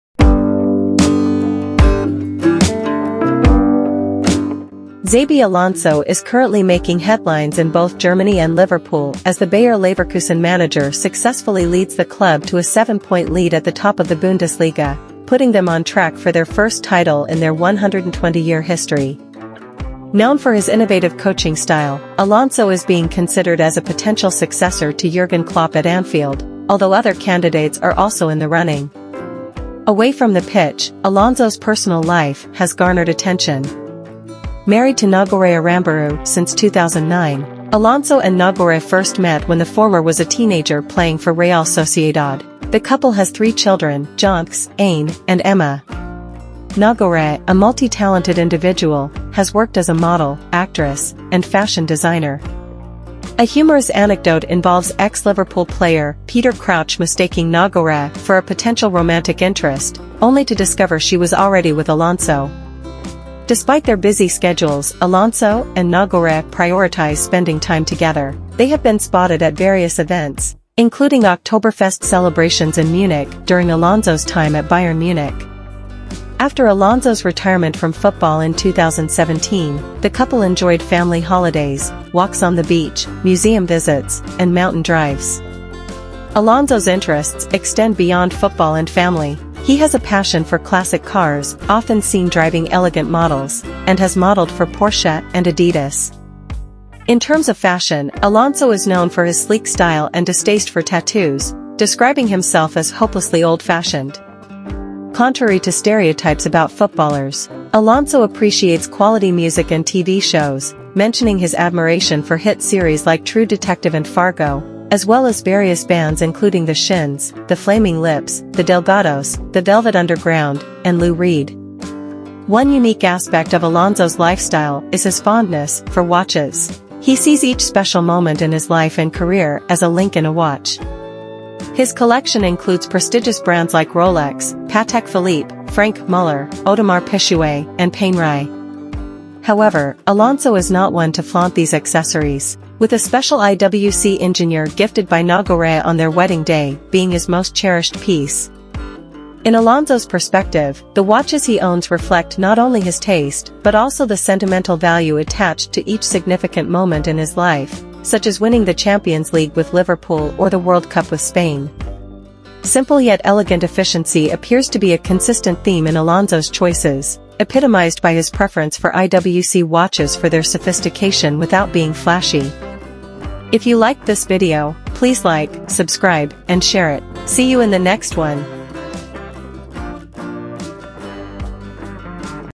Away from the pitch, Xabi Alonso's personal life has garnered attention
Xabi Alonso is currently making headlines in both Germany and Liverpool as the Bayer Leverkusen manager successfully leads the club to a seven-point lead at the top of the Bundesliga, putting them on track for their first title in their 120-year history.

Known for his innovative coaching style, Alonso is being considered as a potential successor to Jurgen Klopp at Anfield, although other candidates are also in the running.

Away from the pitch, Alonso's personal life has garnered attention. Married to Nagore Aranburu since 2009, Alonso and Nagore first met when the former was a teenager playing for Real Sociedad. The couple has three children, Jontxe, Ane, and Emma.